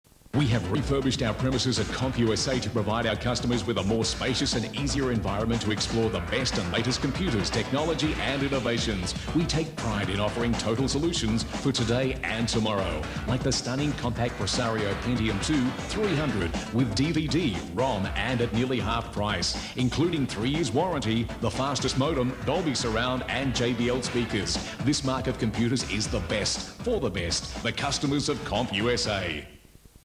Triforce CompUSA Wollongong - 30 Second Ad (August 1998)
Possbily the only CompUSA store in Australia? Recorded off Channel Ten on Monday the 3rd of August 1998.

Equipment:
Ripped from a Polaroid E-180 VHS Tape recorded in SP Speed and Mono audio.
Captured on a Panasonic DMR-BWT955 Hard Drive Recorder in XP quality. Edited and Encoded on VideoReDo TVSuite and VirtualDub.